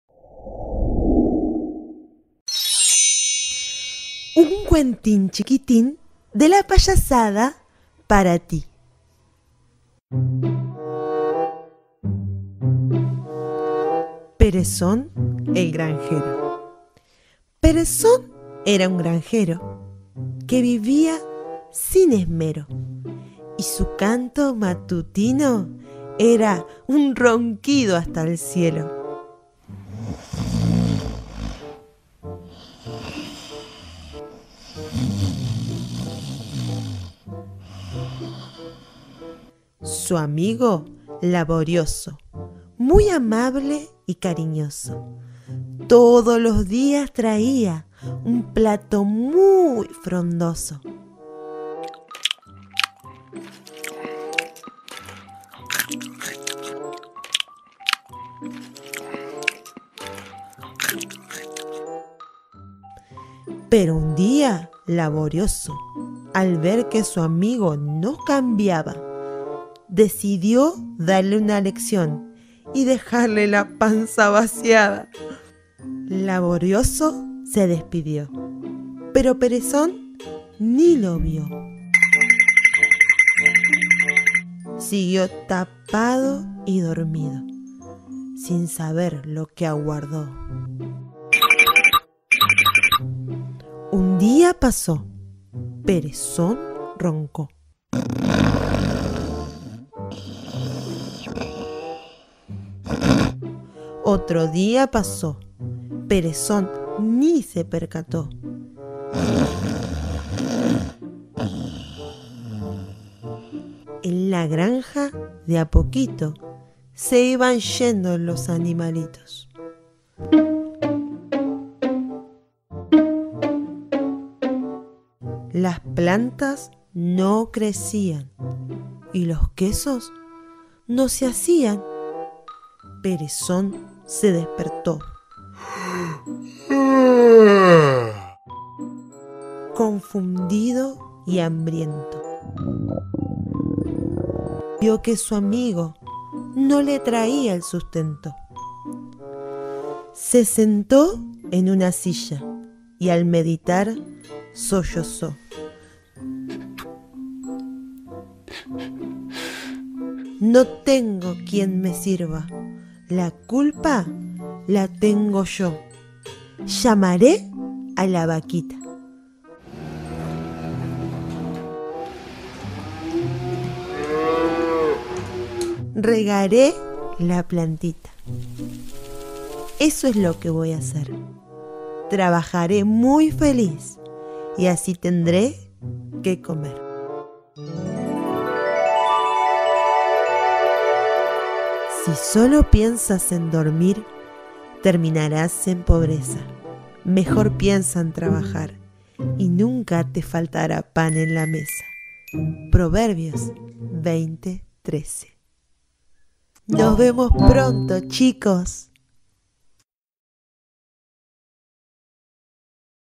Un cuentín chiquitín - Episodio 1: "EL GRANJERO PEREZON"
"Un cuentín chiquitín de  la Payasada para ti" Episodio 1
La nueva serie de "La Payasada de Buenas Nuevas" realizada para todos los niños, inspirado en sabios proverbios que nos enseñan a ser mejores.

En el primer episodio conoceremos al Granjero Perezon y su amigo Laborioso.

Licencia:
El granjero Perezon por Jorge y Gisele Mote se distribuye bajo una Licencia Creative Commons Atribución – No Comercial – Sin Obra Derivada 4.0 Internacional.